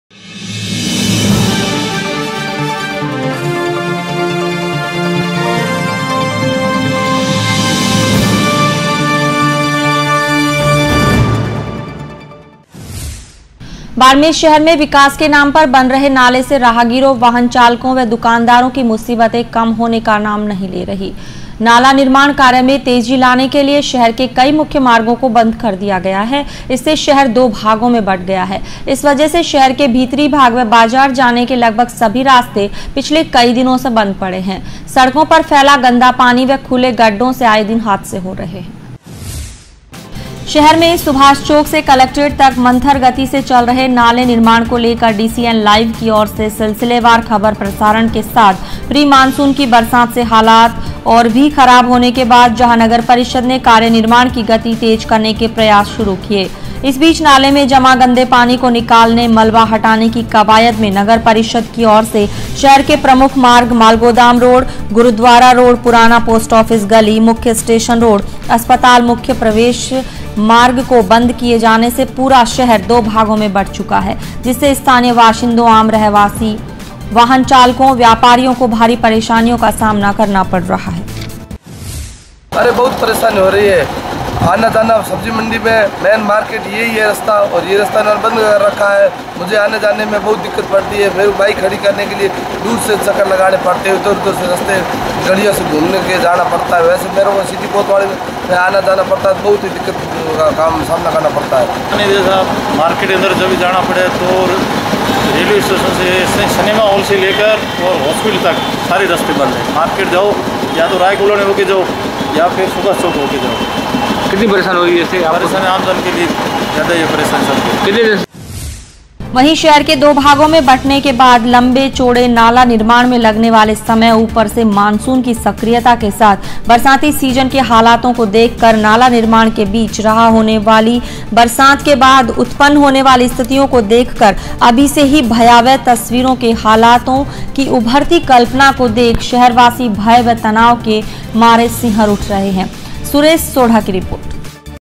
DCN LIVE - नाला निर्माण से बढ़ी मुसीबत
- नाला निर्माण से बढ़ी मुसीबत
- दो भागों में बंटा बाड़मेर शहर
- गड्ढे खोदने से बंद है मुख्य मार्ग
- पिछले कई दिनों से बंद है मुख्य मार्ग
- शहरवासियों को उठानी पड़ रही परेशानी